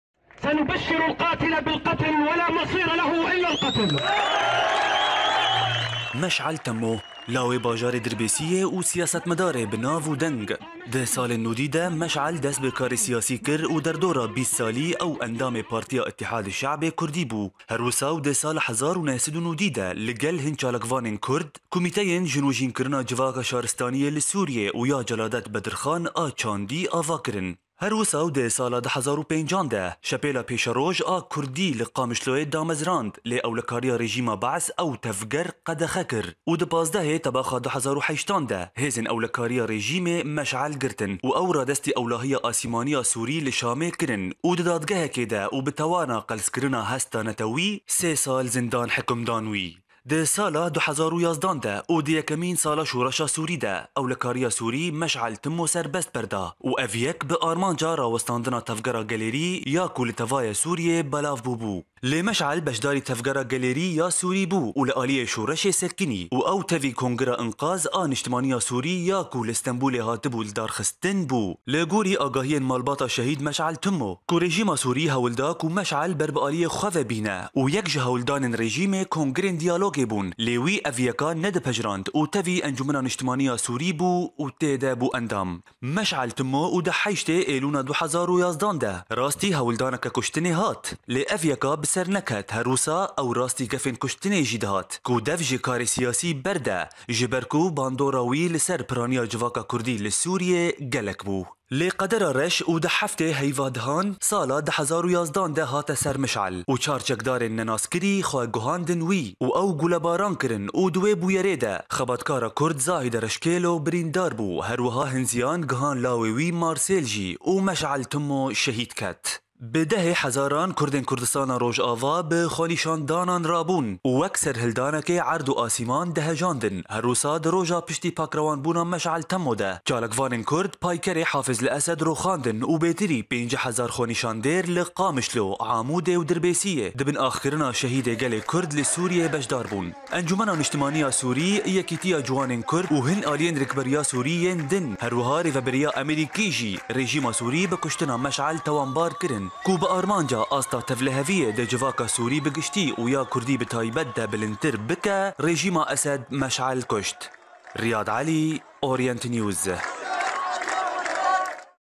5mîn salvegera pakrewanbûna şehîd Meş'el Temo
Wek îro, di sala 2011-an de, serkirdeyê Kurd Meş'el Temo bi destên rûgirtî û çekdaran pakrewan bû.